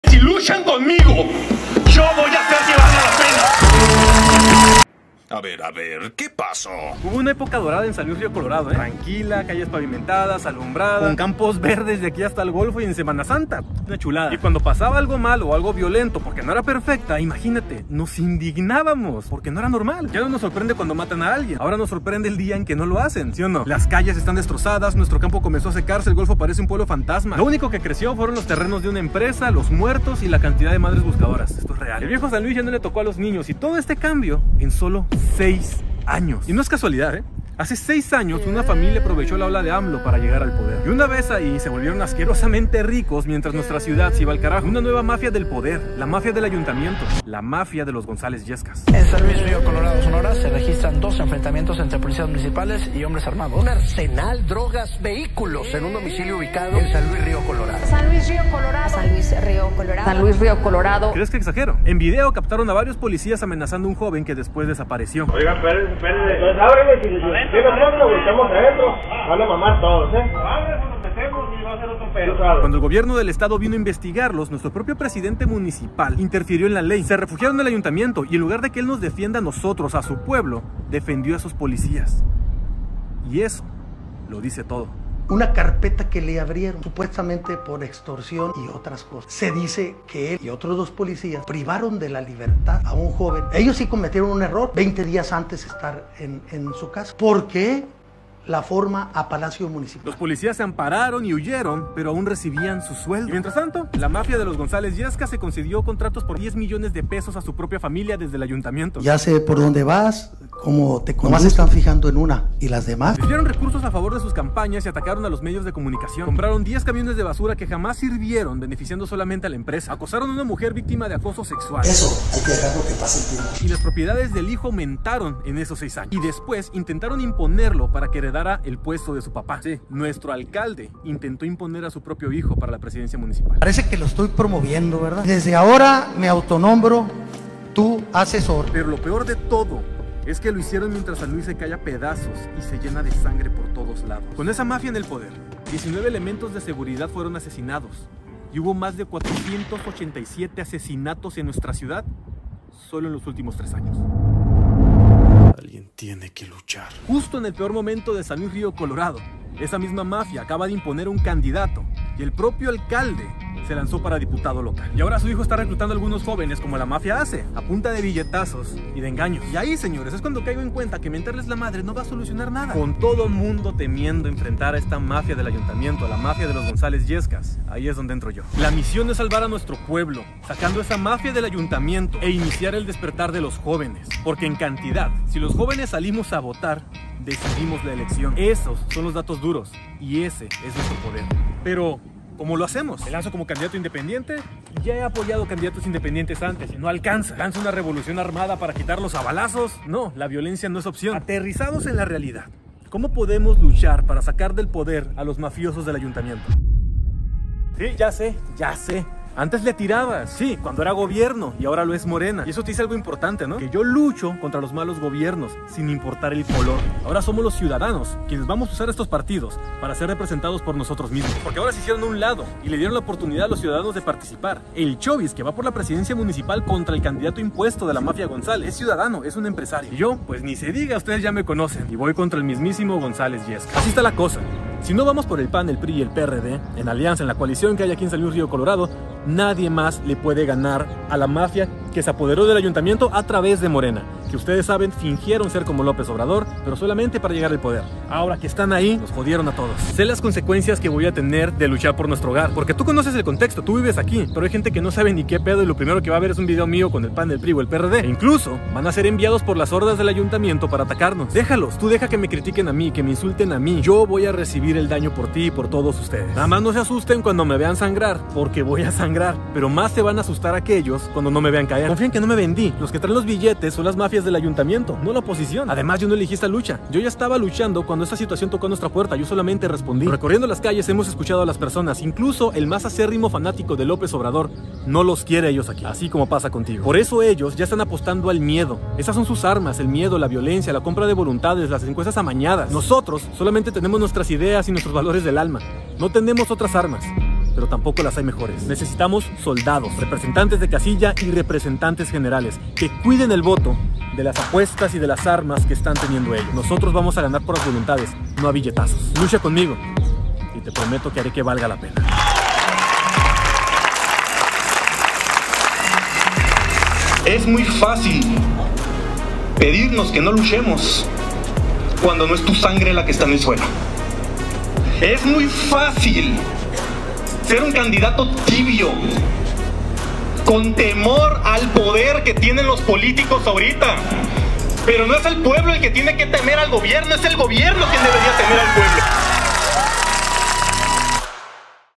LA VERDAD SOBRE MI CANDIDATURA 👀
LA VERDAD SOBRE LA CANDIDATURA DEL WASAPRAKA 👀

A muchos les sorprendió que me lanzara para diputado de mi ciudad, hasta que escucharon el contexto en el que vive mi pueblo. La misión de esta lucha es sacar a la mafia del ayuntamiento para devolver la tranquilidad y dignidad a los Sanluisinos, despertando el espíritu de lucha y hablando con la verdad 👊🏻